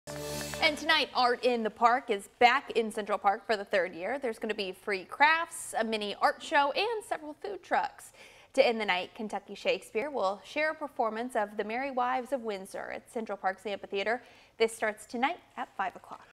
'Art in the Park' is back in Louisville
The event will start at 5 p.m. in Central Park for the third year and will include free crafts, a mini art show, food trucks and a Ky. Shakespeare performance.